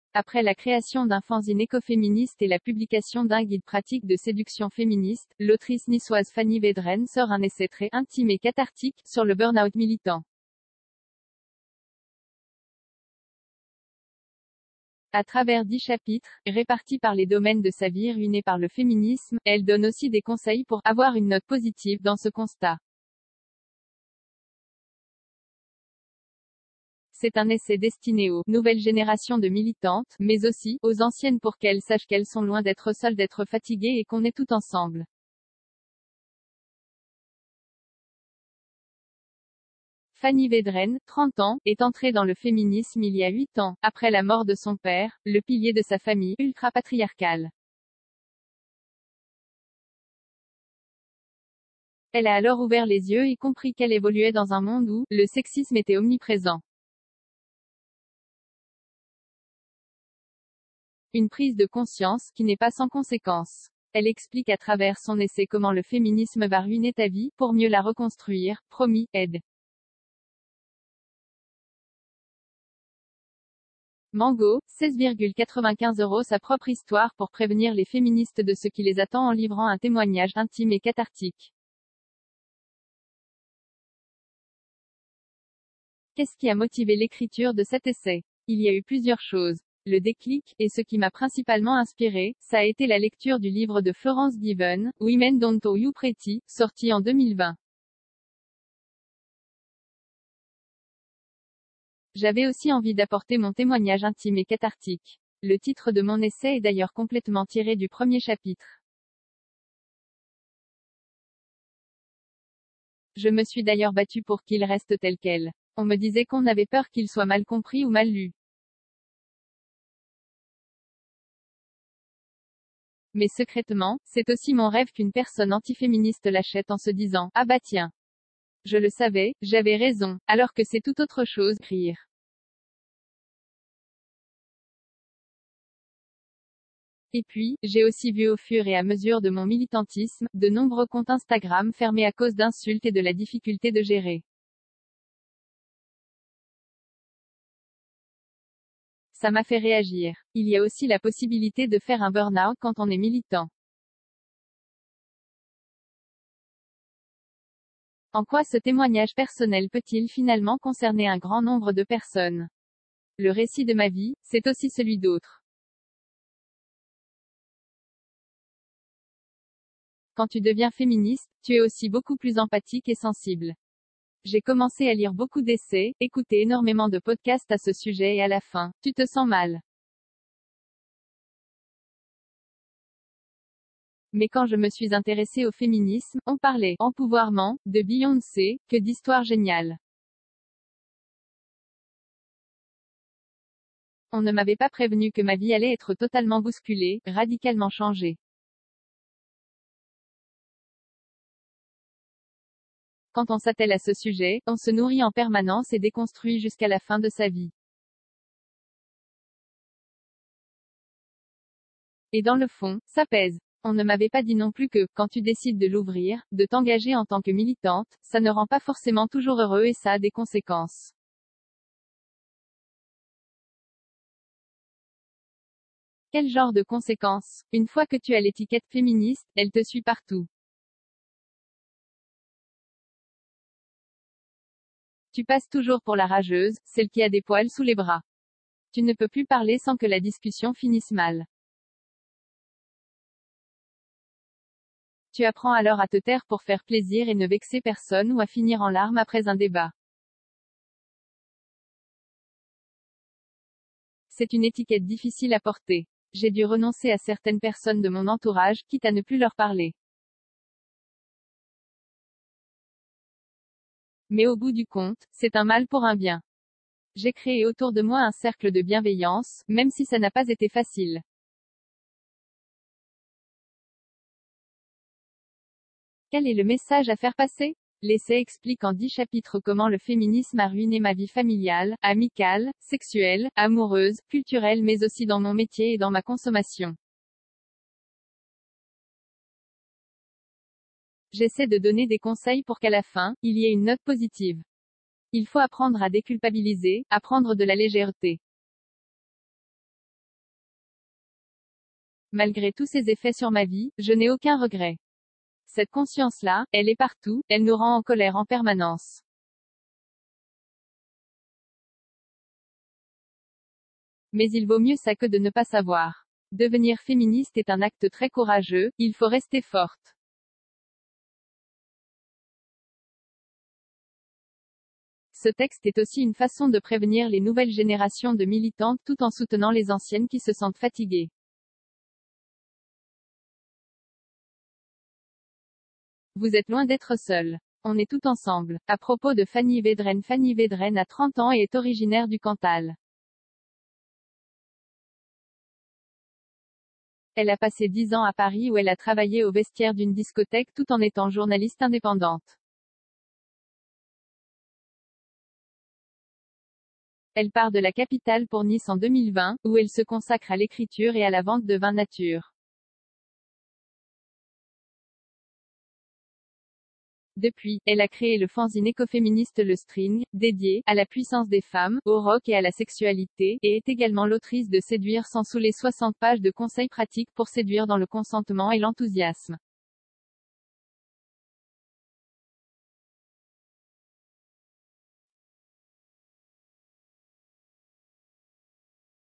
Nice : « Etre féministe, ça a des conséquences », lance Fanny Vedreine, autrice de « Comment le fémi
Après la création d’un fanzine écoféministe et la publication d’un guide pratique de séduction féministe, l’autrice niçoise Fanny Vedreine sort un essai très « intime et cathartique » sur le burn-out militant.A travers dix chapitres, répartis par les domaines de sa vie ruinés par le féminisme, elle donne aussi des conseils pour « avoir une note positive » dans ce constat.C’est un essai destiné aux « nouvelles générations de militantes » mais aussi « aux anciennes pour qu’elles sachent qu’elles sont loin d’être seules d’être fatiguées et qu’on est toutes ensemble ».   Fanny Vedreine, 30 ans, est entrée dans le féminisme il y a huit ans, après la mort de son père, le pilier de sa famille « ultra-patriarcale ». Elle a alors ouvert les yeux et compris qu’elle évoluait dans un monde où « le sexisme était omniprésent ». Une prise de conscience « qui n’est pas sans conséquences ». Elle explique à travers son essai Comment le féminisme va ruiner ta vie (pour mieux la reconstruire, promis !) -ed. Mango, 16,95 euros- sa propre histoire pour prévenir les féministes de ce qui les attend en livrant un témoignage « intime et cathartique ». Qu’est-ce qui a motivé l’écriture de cet essai ? Il y a eu plusieurs choses. Le déclic, et ce qui m’a principalement inspirée, ça a été la lecture du livre de Florence Given, Women don’t owe you pretty, sorti en 2020. J’avais aussi envie d’apporter mon témoignage intime et cathartique. Le titre de mon essai est d’ailleurs complètement tiré du premier chapitre. Je me suis d’ailleurs battue pour qu’il reste tel quel. On me disait qu’on avait peur qu’il soit mal compris ou mal lu. Mais secrètement, c’est aussi mon rêve qu’une personne antiféministe l’achète en se disant « ah bah tiens ! Je le savais, j’avais raison », alors que c’est tout autre chose (rires). Et puis, j’ai aussi vu au fur et à mesure de mon militantisme, de nombreux comptes Instagram fermer à cause d’insultes et de la difficulté de gérer. Ça m’a fait réagir. Il y a aussi la possibilité de faire un burn-out quand on est militant. En quoi ce témoignage personnel peut-il finalement concerner un grand nombre de personnes ? Le récit de ma vie, c’est aussi celui d’autres. Quand tu deviens féministe, tu es aussi beaucoup plus empathique et sensible. J’ai commencé à lire beaucoup d’essais, écouter énormément de podcasts à ce sujet et à la fin, tu te sens mal. Mais quand je me suis intéressée au féminisme, on parlait « empouvoirement », de Beyoncé, que d’histoires géniales. On ne m’avait pas prévenue que ma vie allait être totalement bousculée, radicalement changée. Quand on s’attelle à ce sujet, on se nourrit en permanence et déconstruit jusqu’à la fin de sa vie. Et dans le fond, ça pèse. On ne m’avait pas dit non plus que, quand tu décides de l’ouvrir, de t’engager en tant que militante, ça ne rend pas forcément toujours heureux et ça a des conséquences. Quel genre de conséquences ?    Une fois que tu as l’étiquette « féministe », elle te suit partout. Tu passes touj